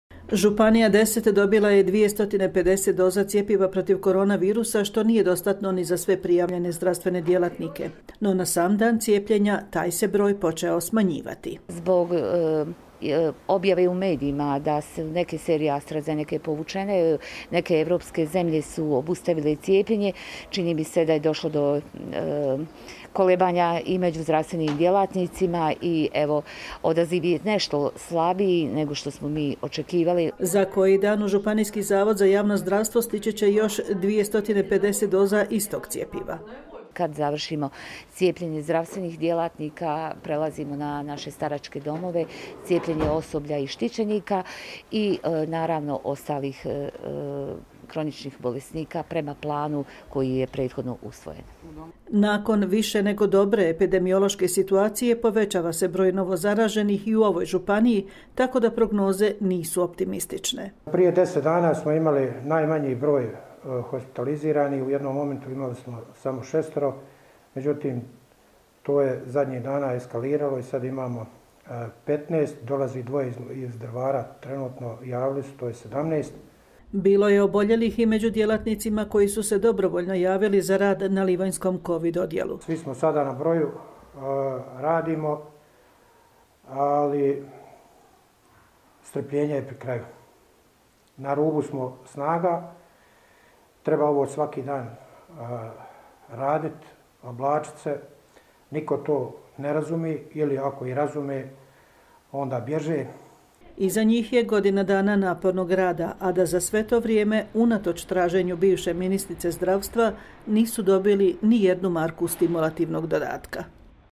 Neočekivano slab odaziv za imunizaciju zdravstvenih radnika Kantona 10
Danas je u Kantonu 10 počela imunizacija zdravstvenih djelatnika u zdravstvenim ustanovama na području svih šest lokalnih samouprava. Radi se o cjepivu koje je donirala Republika Srbija, a koje je Federalni Zavod za javno zdravstvo rasporedio po svim kantonima.

Kanton 10 dobila je 250 doza cjepiva protiv koronavirusa, što nije dostatno ni za sve prijavljene zdravstvene djelatnike. No, na sam dan cijepljenja taj se broj počeo smanjivati.
federalna.ba